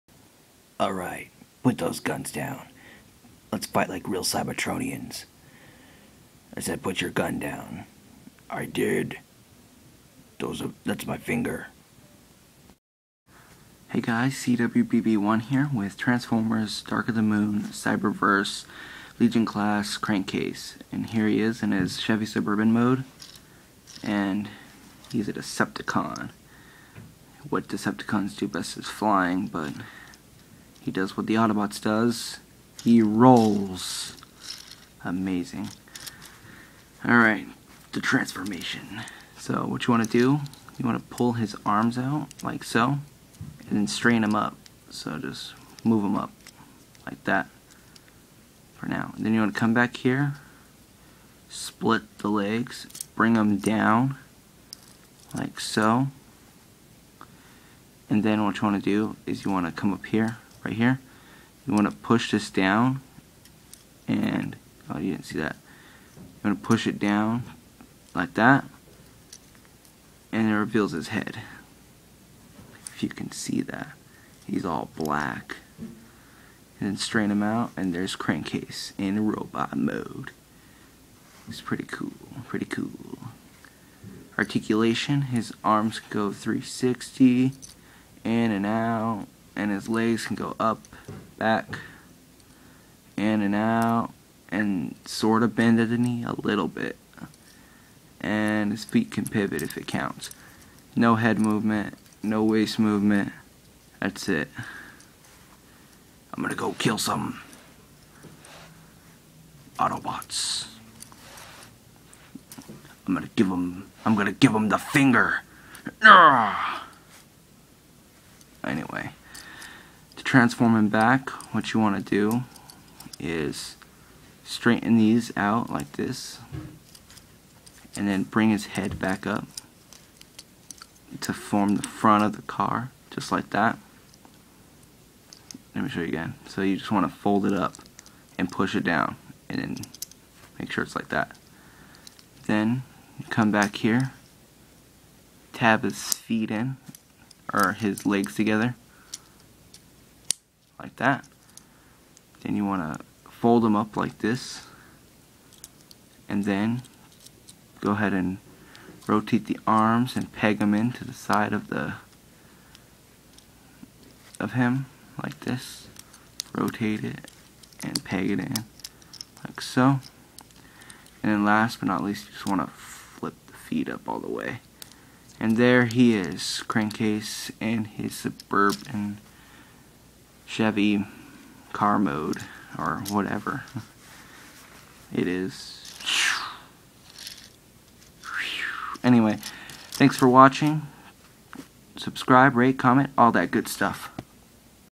Transformers DOTM cyberverse Crankcase Toy Review!
Review of transformers DOTM cyberverse legion class crankcase